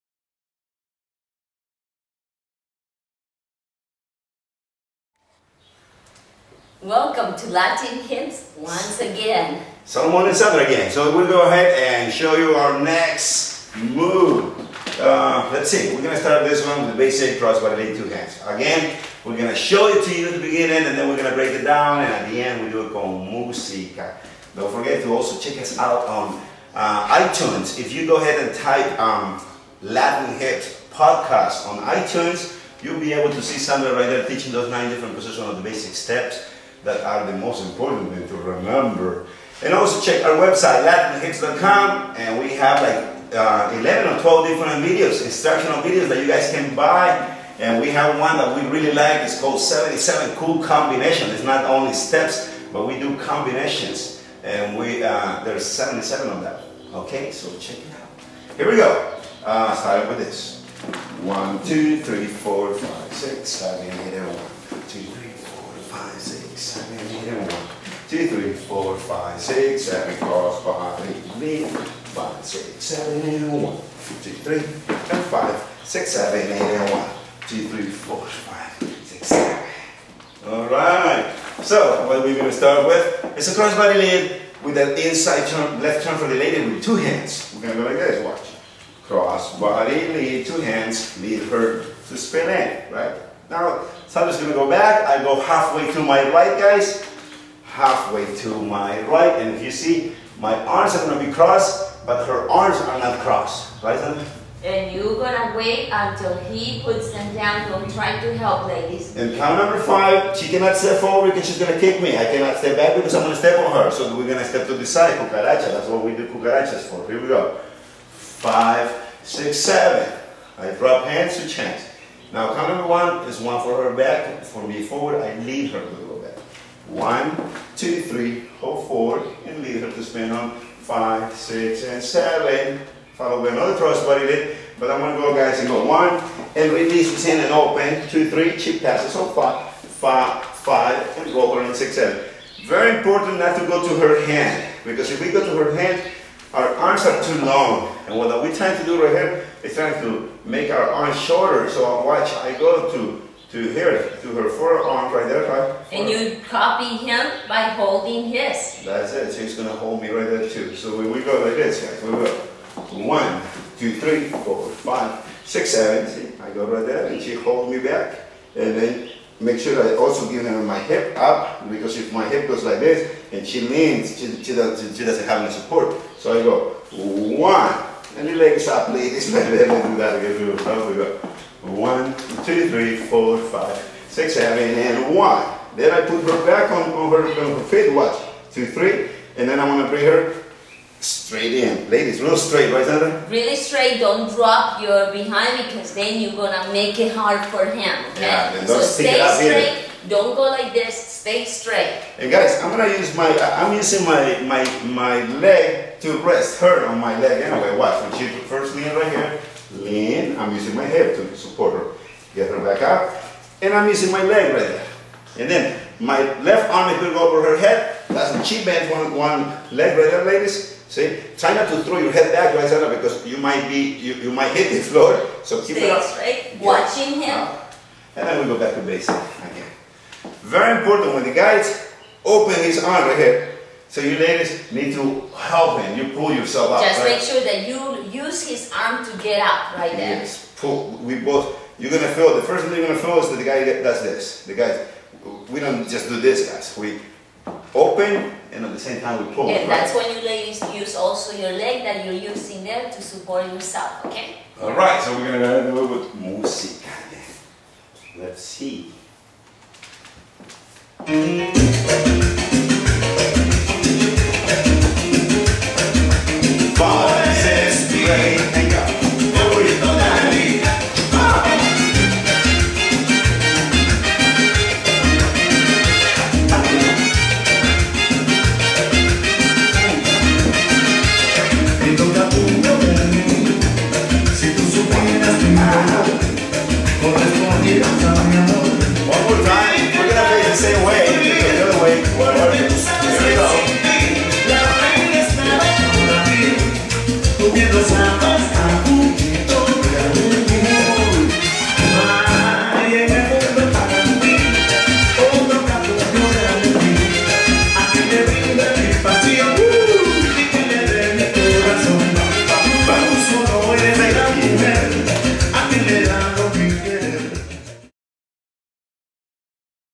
Salsa Class By Salomon & Sandra Rivera, Three Falls Dip
Salomon & Sandra Rivera. They have been dancing since the age of eight and six they have spent their entire life developing and perfecting their unique style of Salsa. One of the pioneers dance instructors on the L.A. scene they have been teaching professionally since 1990. Their efforts have been rewarded with invitations to teach all over the world. Together they won the 2001 & 2002 International and US Salsa Championship. Salomon holds over 90 championships. He has been the subject of magazine covers, featured articles in major newspapers and television appearances including Good Morning America and Star Search. Among many titles Salomon holds includes

4 Time International Salsa Champion
4 Time U.S. Salsa Champion
3 Time World Salsa Champion